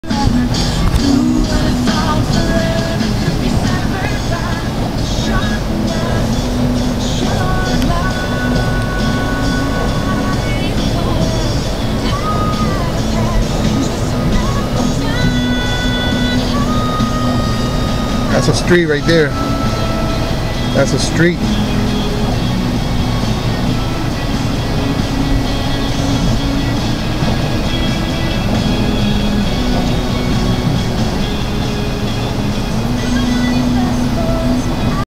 hurricane irene rocks philly
schuylkill river floods the streets